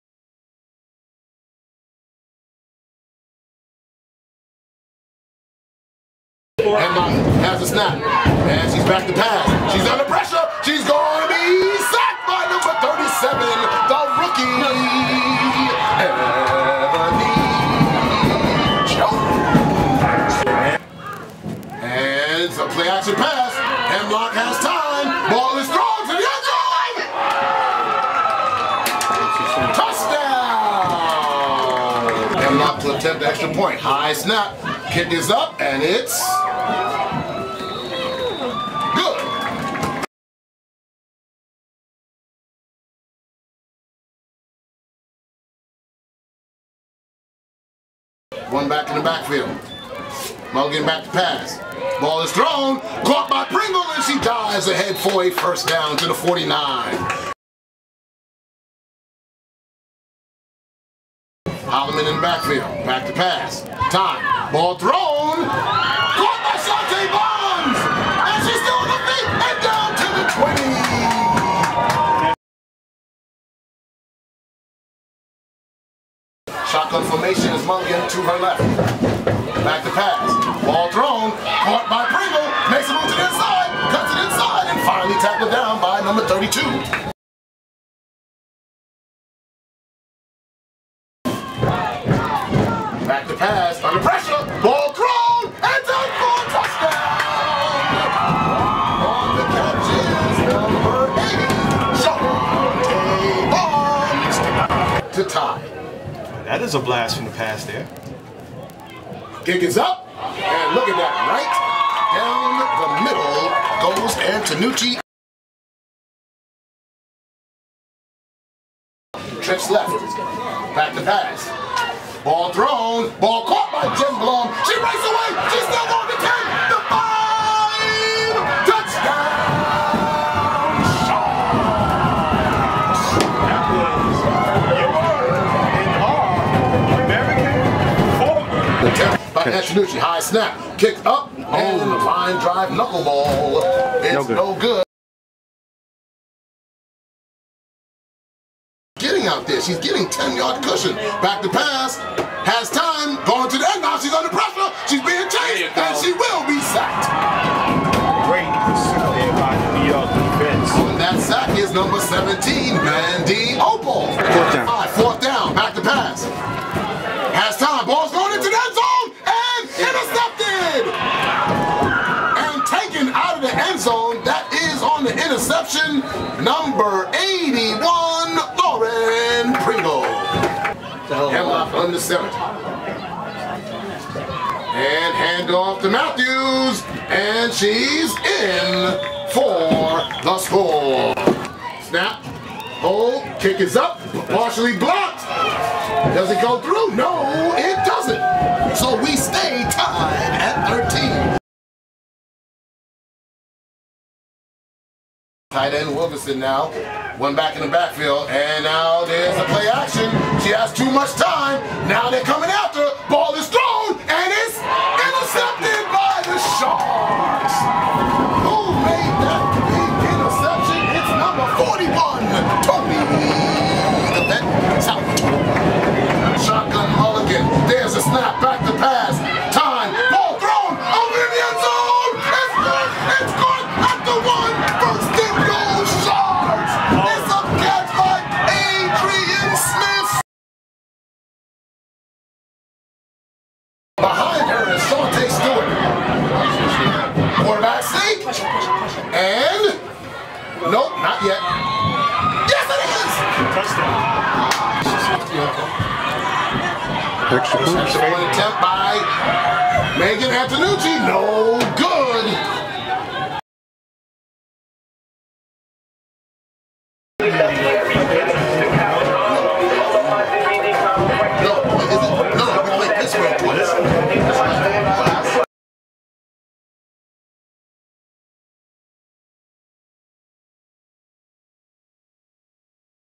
2010 #2 - D.C. Divas at New York Sharks - Women's Pro Football
Women's pro footballl highlights from the IWFL contest between the D.C. Divas and the New York Sharks. New York came from behind to win by the score of 19-13.  The winning score coming with less than 2 minutes left to go in the game.  This game was the Sharks 12th home opener.  The football game was played on April17th, 2010.  WFNradio's Pat Brown provides the play-by-play.